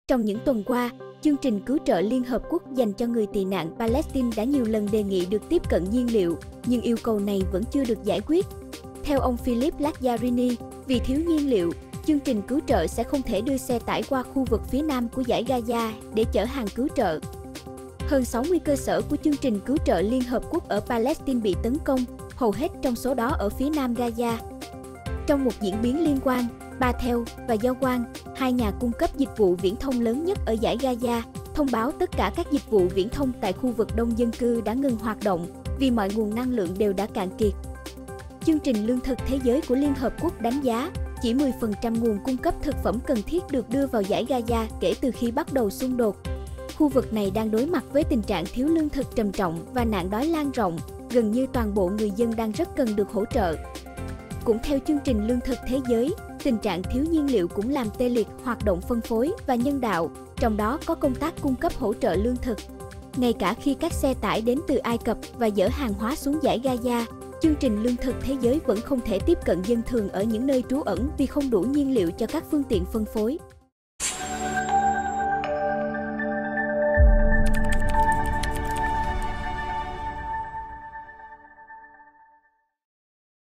Chương trình Cứu trợ Liên hợp quốc có thể ngừng hoạt động ở Dải Gaza
Trong những tuần qua, Chương trình Cứu trợ Liên hợp quốc dành cho người tị nạn Palestine đã nhiều lần đề nghị được tiếp cận nhiên liệu, nhưng yêu cầu này vẫn chưa được giải quyết. Theo ông Philippe Lazzarini, vì thiếu nhiên liệu, chương trình cứu trợ sẽ không thể đưa xe tải qua khu vực phía Nam của Dải Gaza để chở hàng cứu trợ.